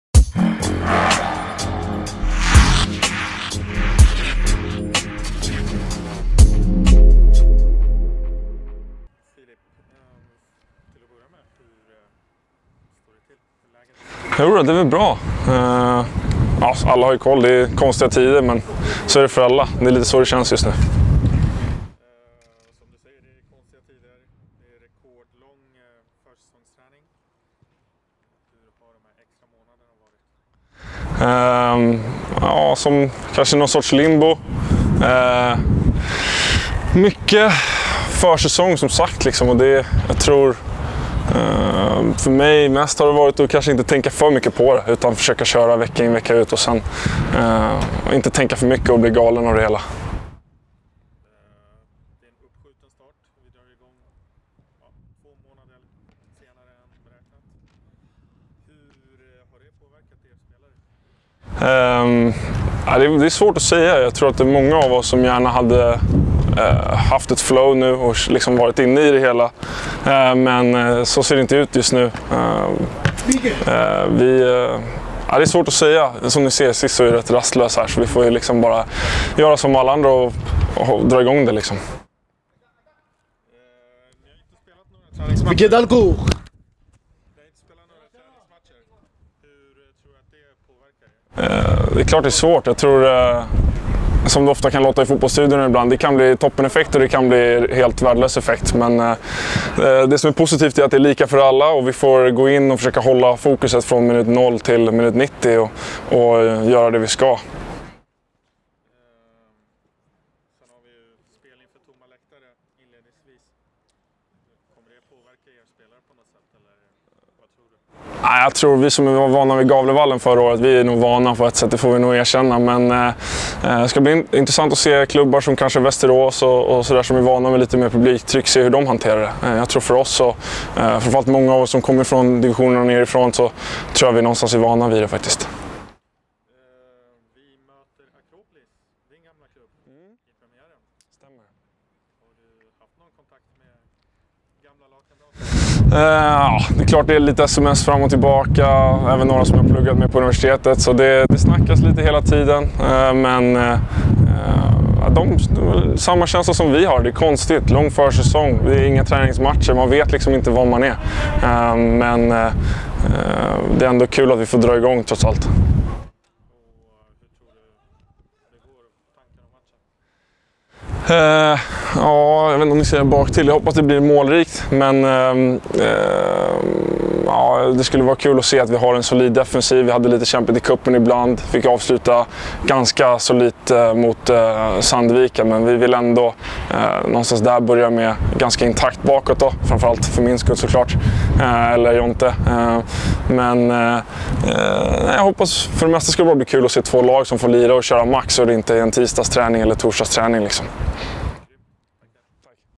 Intervju med Filip Anger inför Superettan-premiären
Målvakten Filip Anger intervjuades inför den stundande Superettan-premiären om den rekordlånga försäsongen, en uppskjutna premiären och tisdagens match mot sin gamla klubb Akropolis IF.